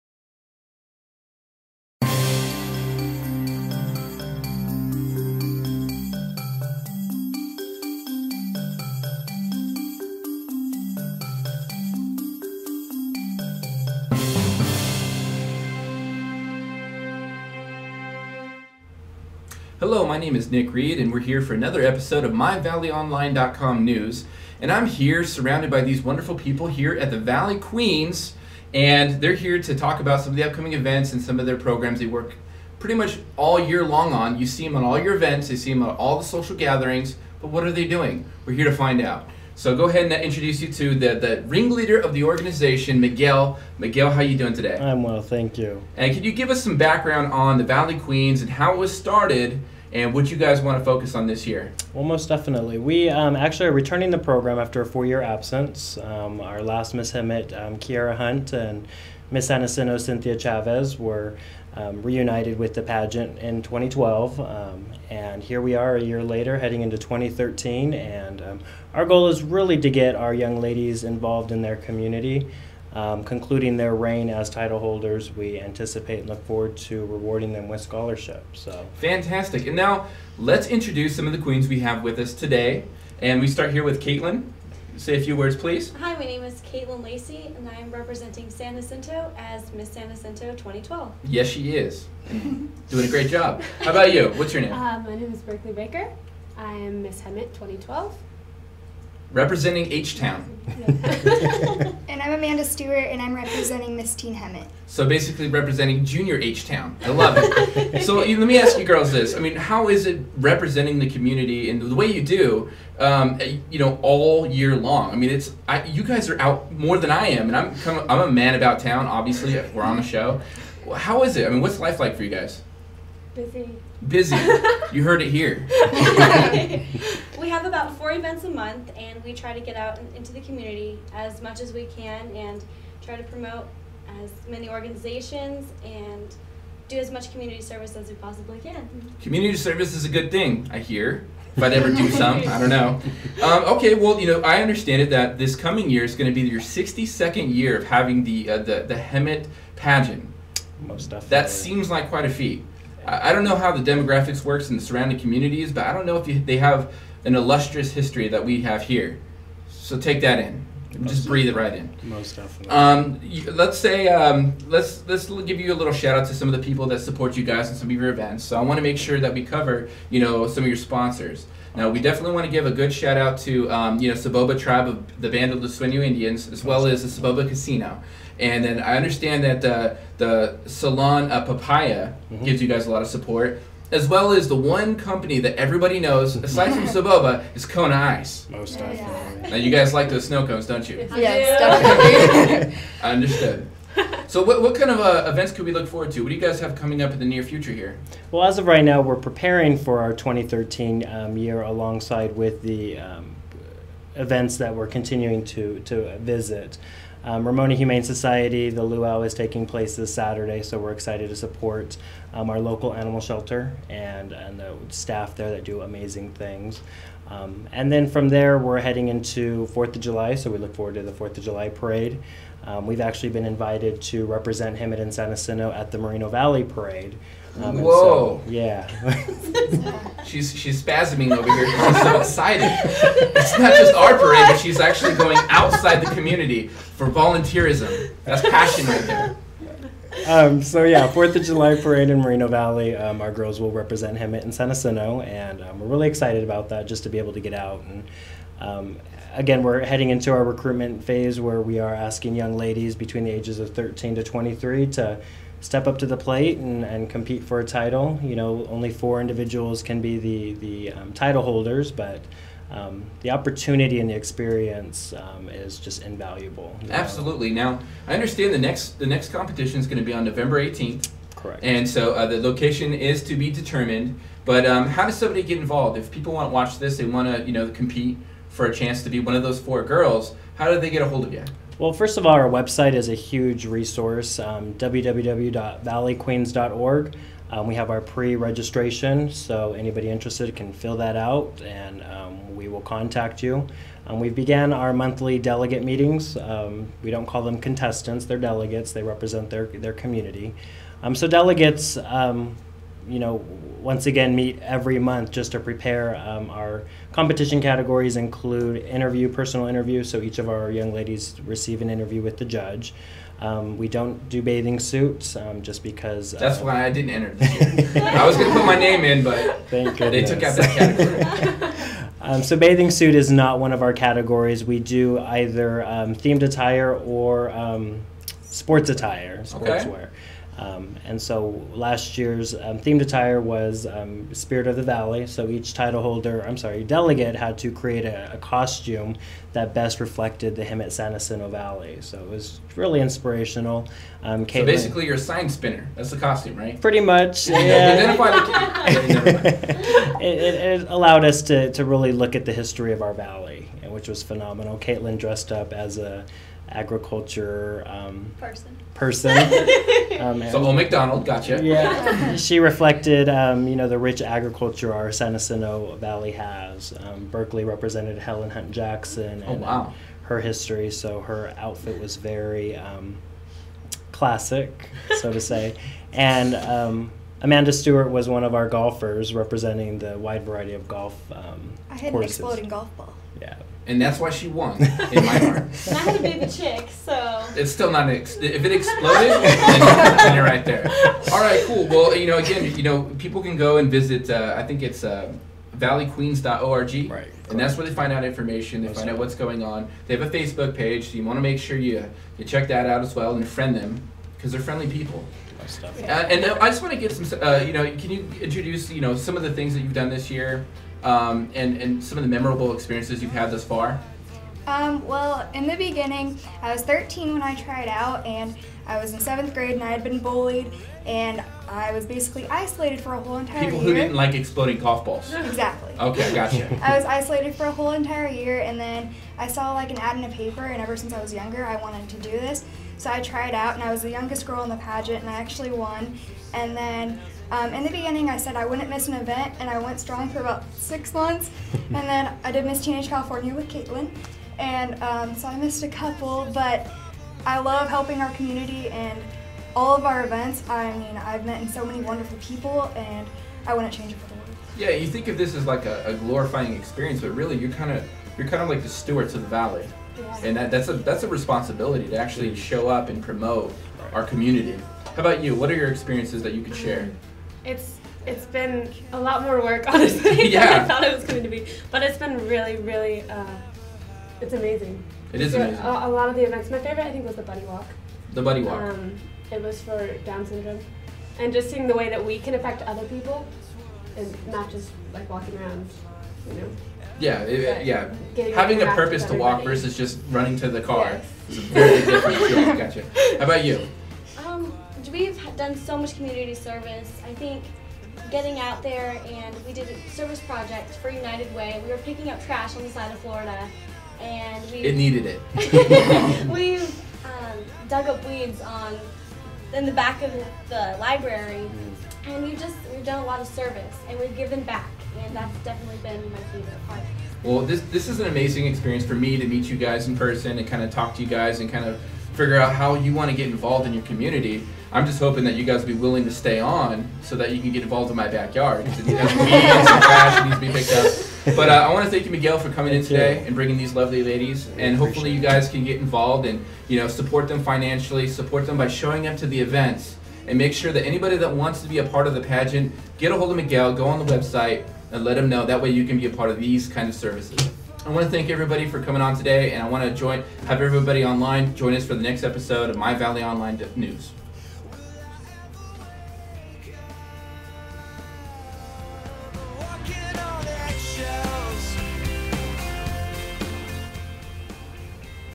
My Valley Online Welcomes The Valley Queens
The reigning Miss Hemet, Miss Teen Hemet and Miss San Jacinto join Nick Reed in the studio for a look into the program.